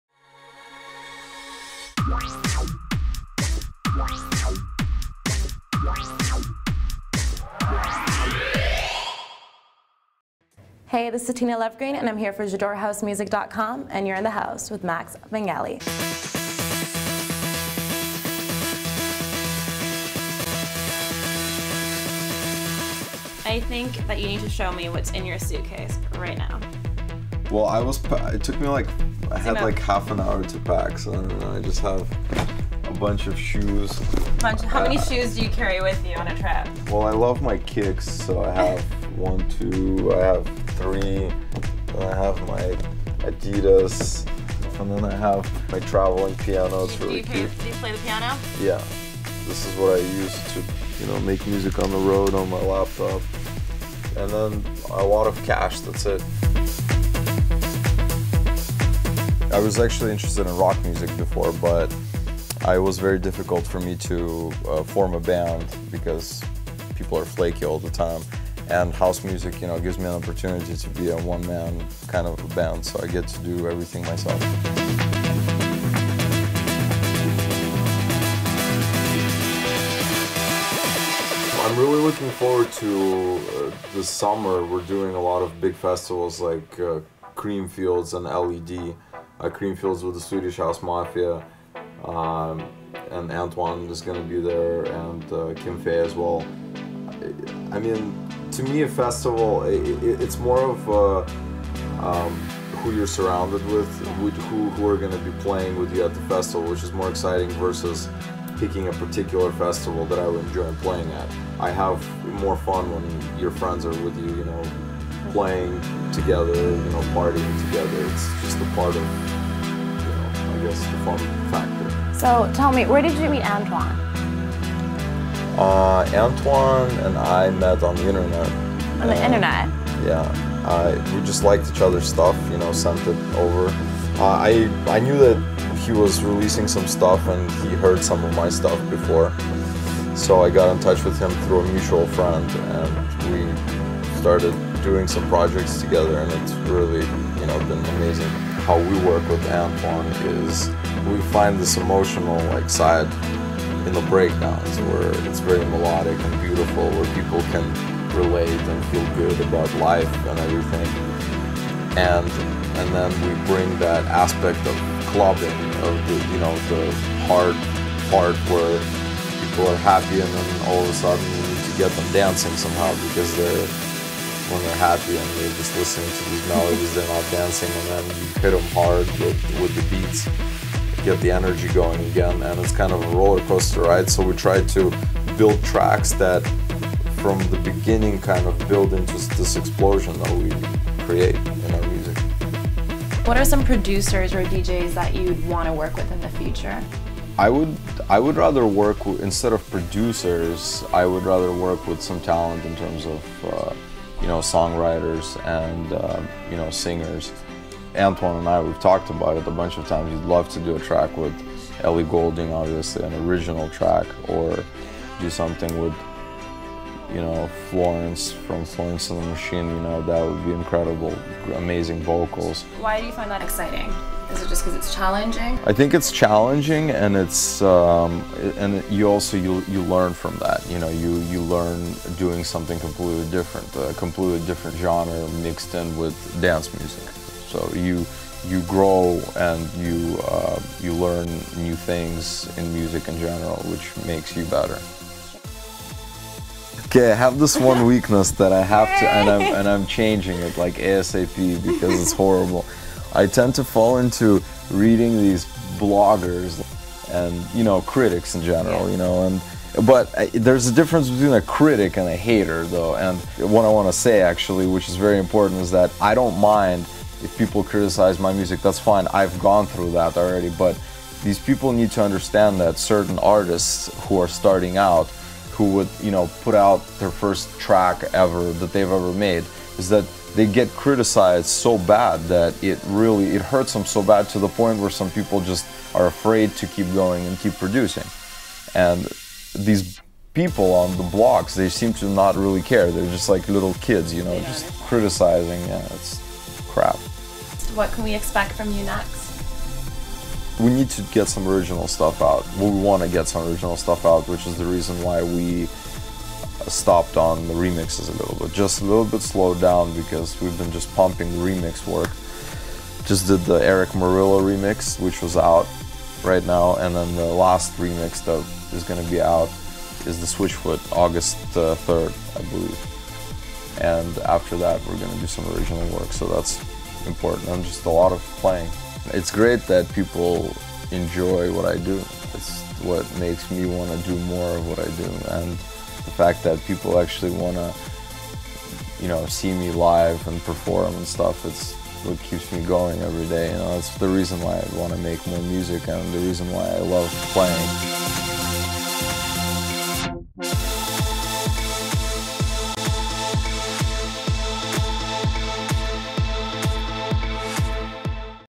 Max Vangeli Interview with JadoreHouseMusic.com
Max talks about how he got introduced to house music, his influences, his relationship with AN21, his future!
A big thanks to Max Vangeli, Denis & MSH Events.